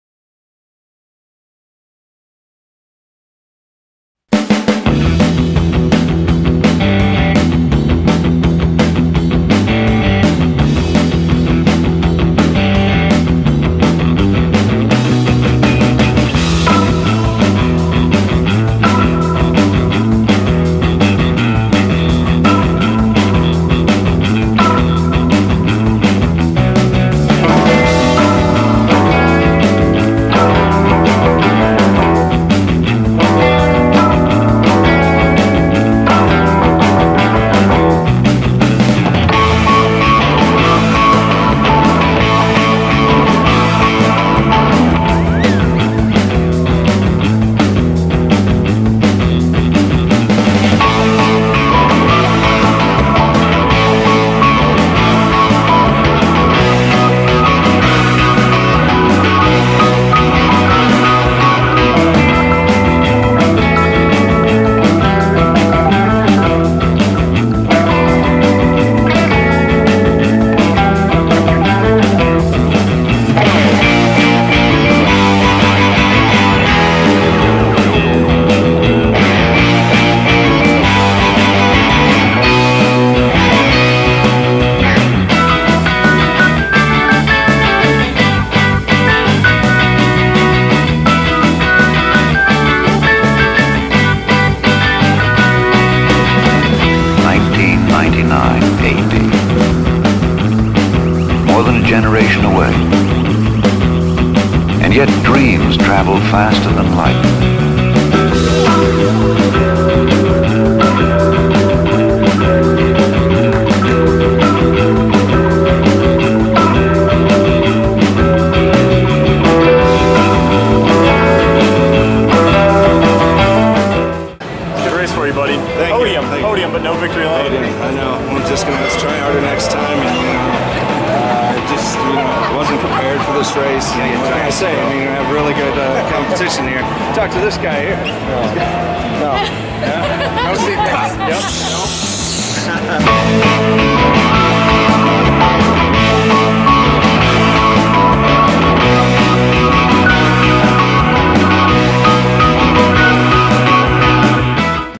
Go Karts with Tucker Smax
Tucker brought some friends to MB2 Raceway in Minneapolis for some electric go-kart action. We raced around the track and zoomed fast and probably bumped into each other a little more than we should have.